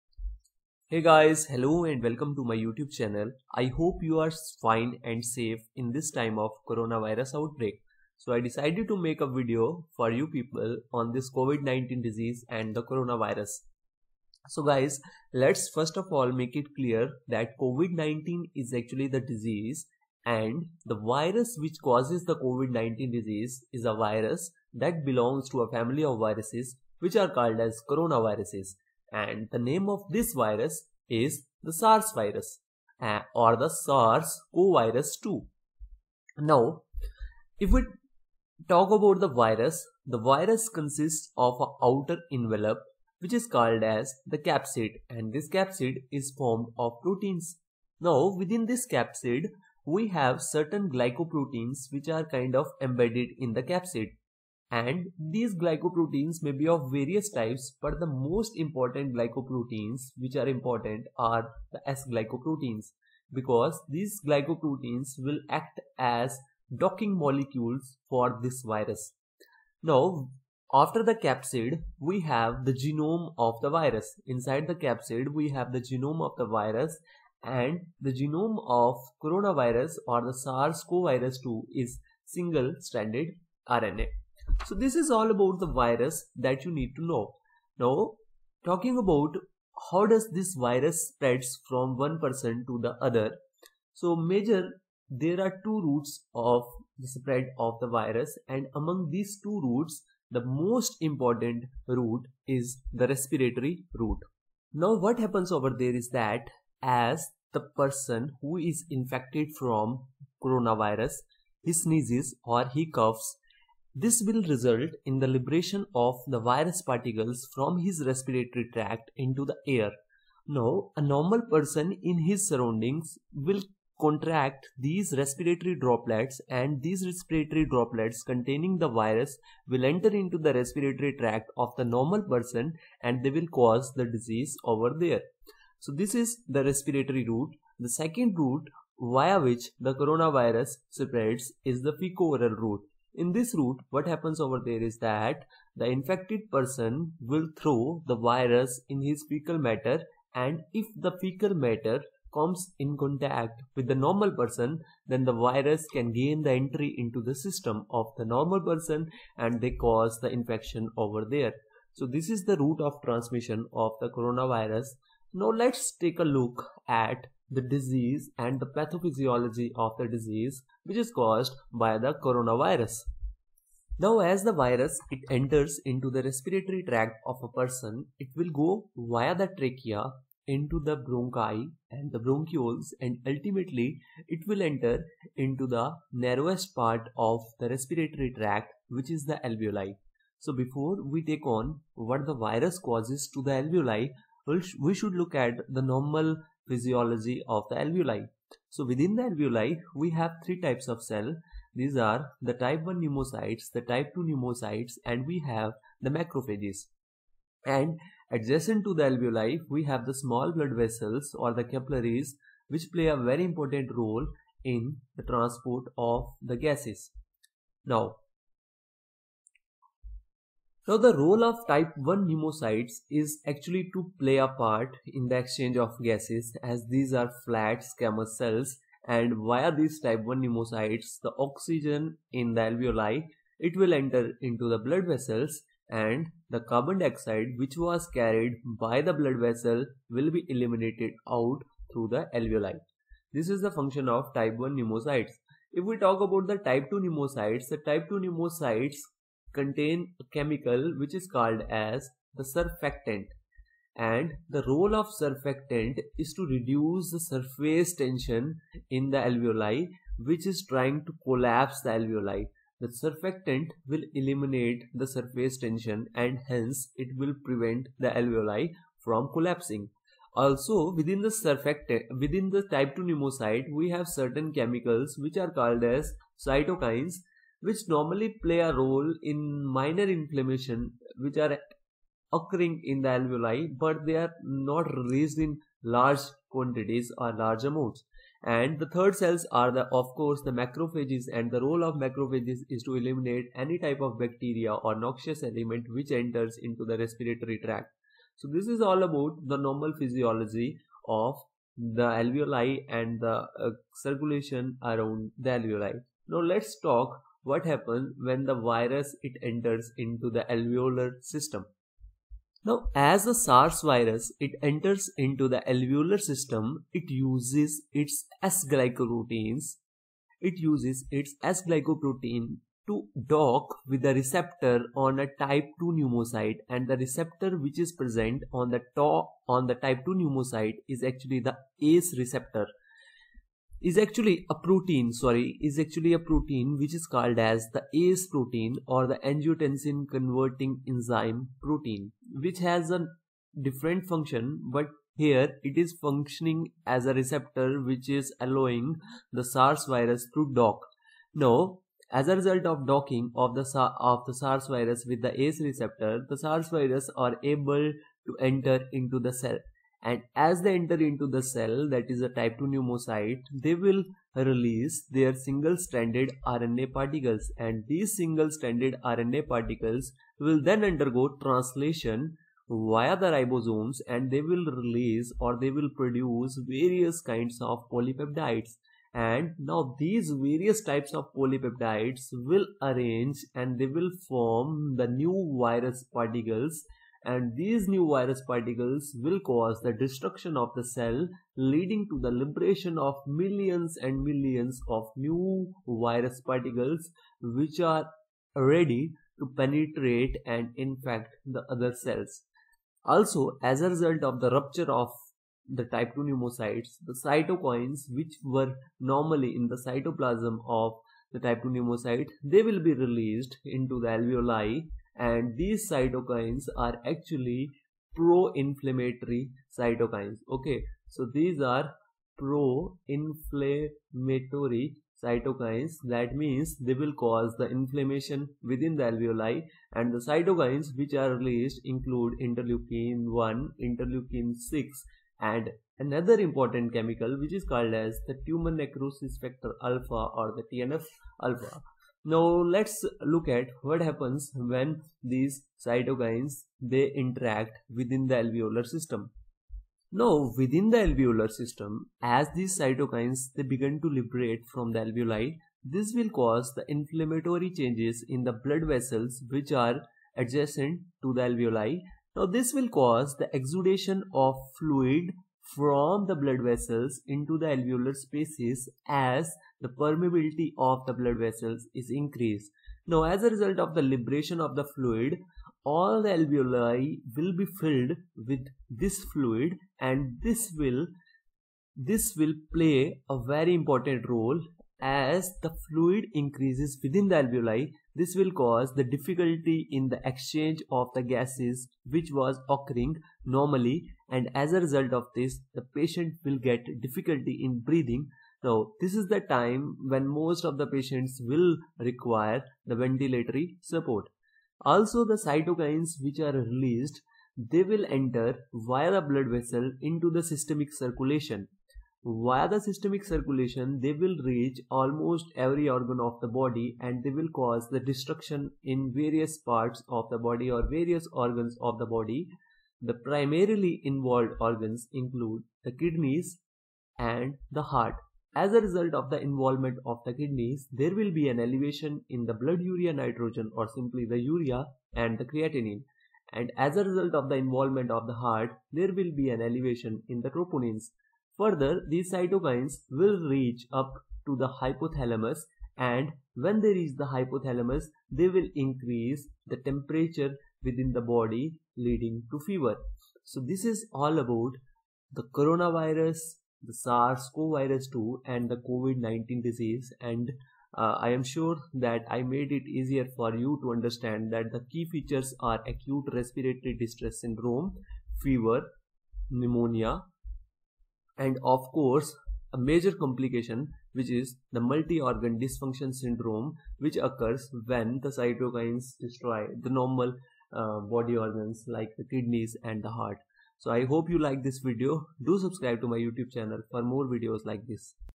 Covid-19 Disease -Pathophysiology and Clinical features
Coronavirus disease 2019 (COVID-19) is an infectious disease caused by severe acute respiratory syndrome coronavirus 2 (SARS-CoV-2).The disease was first identified in 2019 in Wuhan, the capital of China's Hubeiprovince, and has since spread globally, resulting in the 2019–2020 coronavirus pandemic. Common symptoms include fever, cough, and shortness of breath. Other symptoms may include muscle pain, sputumproduction, diarrhea, sore throat, abdominal pain, and loss of smell or taste.While the majority of cases result in mild symptoms, some progress to pneumonia and multi-organ failure. As of March 25, 2020, the overall rate of deaths per number of diagnosed casesis 4.5 percent; ranging from 0.2 percent to 15 percent according to age group and other health problems.